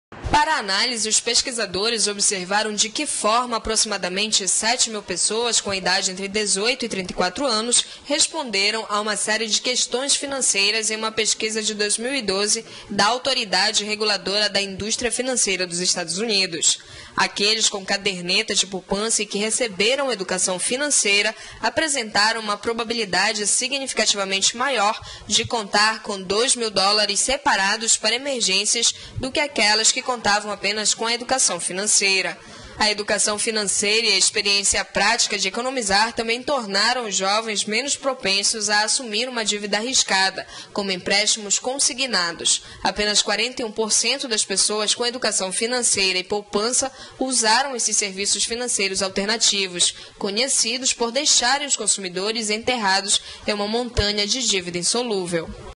Jornal da Amazônia | Pesquisa recomenda que crianças tenham poupança
Pesquisa recomenda que crianças tenham poupança